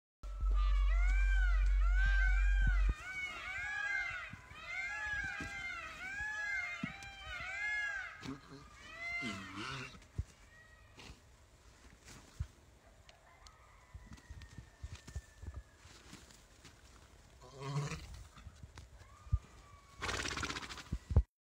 Peacock noises 🦚
if you were ever curious about what peacocks sound like, listen to the beginning of this video again 🦚